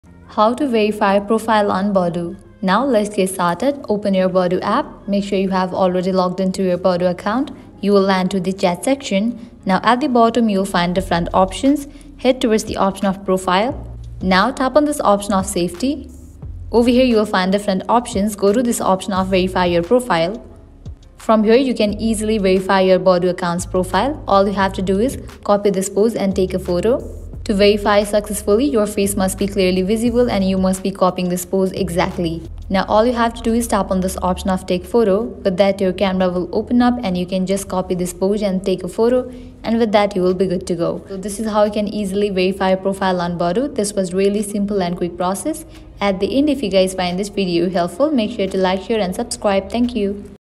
How to Verify Profile on Badoo | 2023
👋 Welcome to Login Giants! In this tutorial, we'll guide you through the process of verifying your profile on Badoo in the year 2023. If you're looking to enhance your profile's credibility and stand out on the platform, you're in the right place. We'll provide you with a step-by-step guide on how to easily verify your profile on Badoo.

In this tutorial, we'll cover the entire process of verifying your Badoo profile. Whether you're interested in increasing your chances of meaningful connections or gaining more visibility, we've got you covered.

Tips for Verifying Your Profile on Badoo
We're here to assist you every step of the way. If you find this tutorial helpful, consider giving the video a thumbs up and sharing it with others who might also want to enhance their Badoo profile.

📱 Timestamps:

0:00 - Introduction
0:06 - How to Verify Profile on Badoo